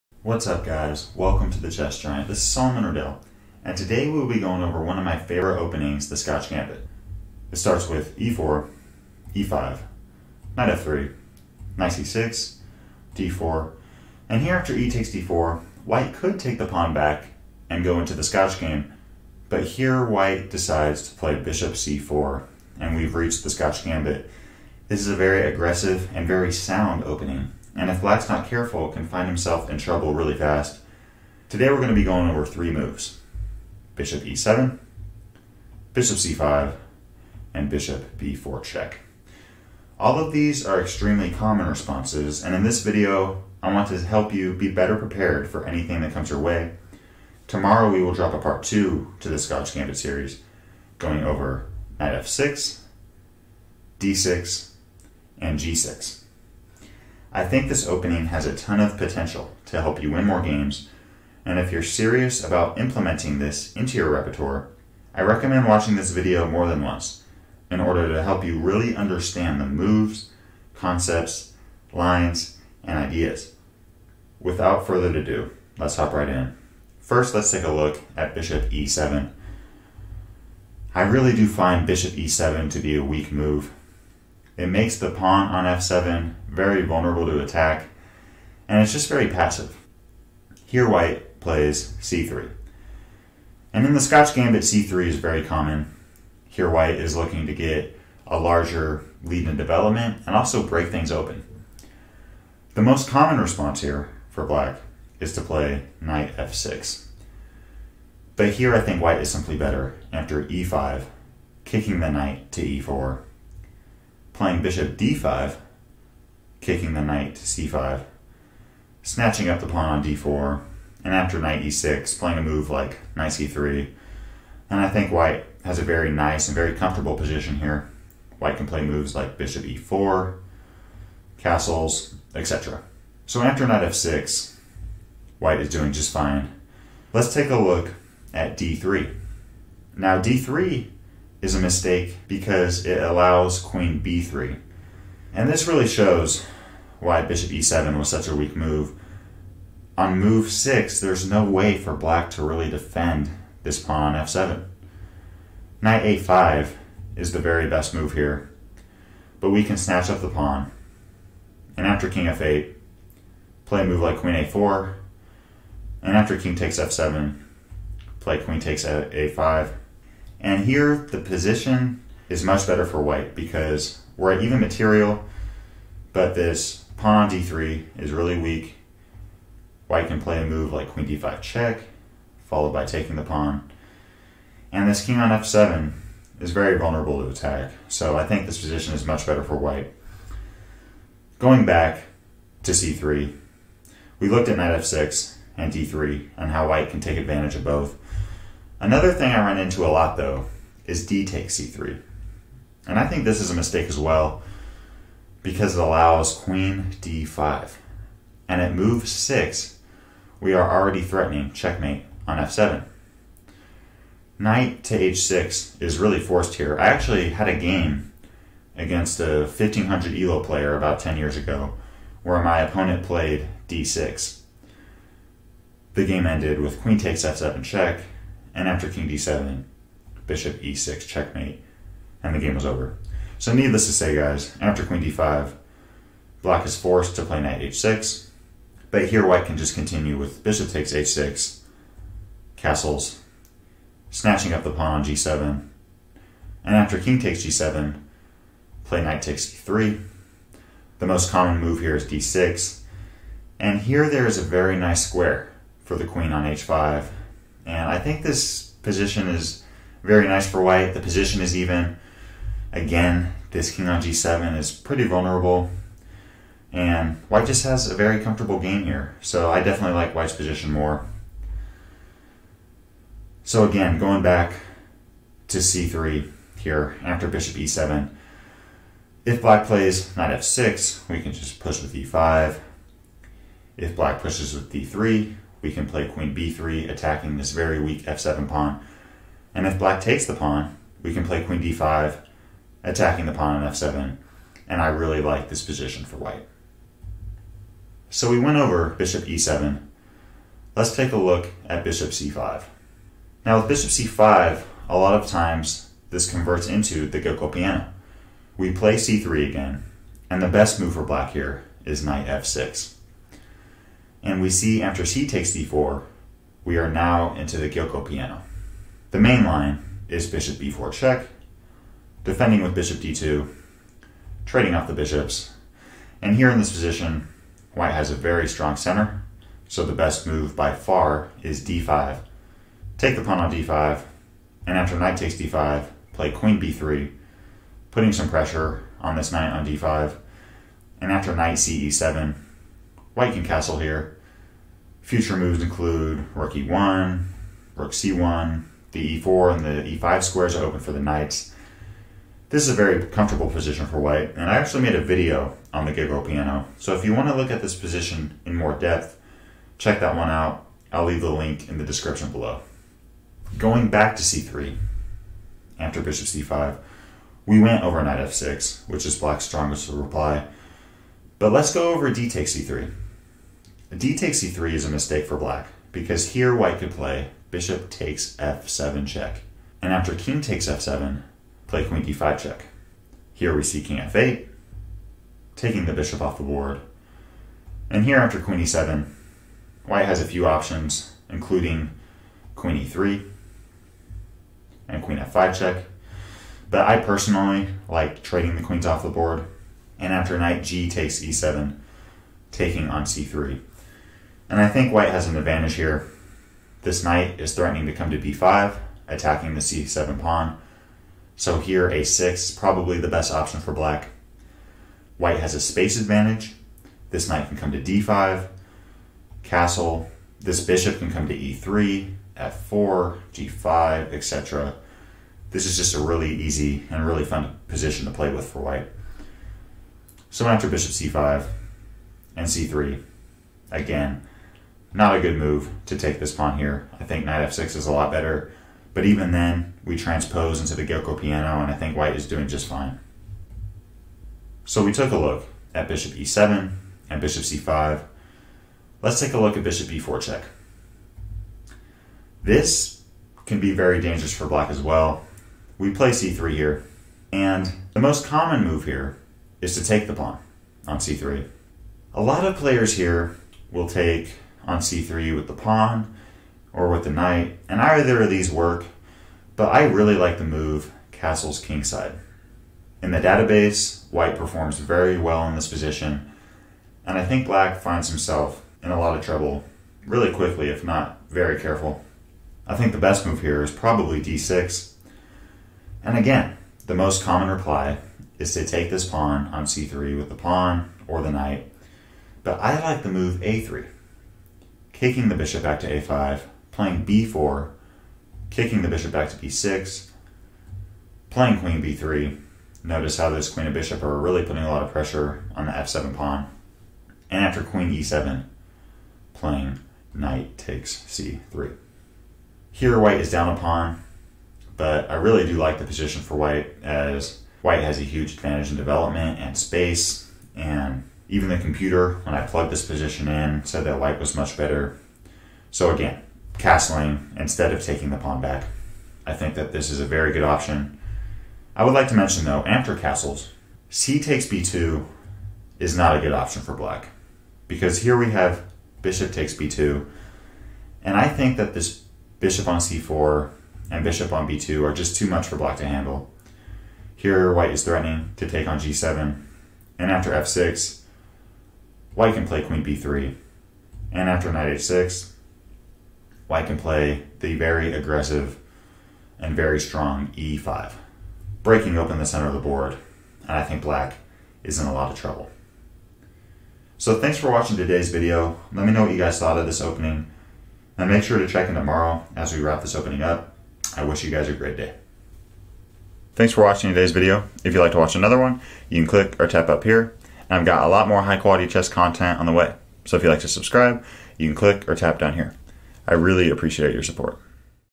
Chess Openings: Learn to Play the Scotch Gambit - Lesson 1
The Scotch Gambit is an aggressive-yet-sound chess opening that has been used and developed for centuries, and I recommend it to anyone who wants to learn an opening strategy that actually works!  Many gambits in chess present high risks, rely on cheap and short threats, and fall apart against strong opposition.  Thorough preparation with the Scotch Gambit, however, can equip you for success against all levels.  Many have strayed from this opening because it has struggled against main lines; in this video, however, I have recommended moves that are not commonly played, yet show excellent results with proven track records at the 2200 ELO level.  This video covers how to best attack the responses of 4. ... Be7, Bc5, and Bb4+.

In the meantime, learn even more about chess openings by visiting our lesson on the Giuoco Piano

Interested in PRIVATE LESSONS? Here's the link!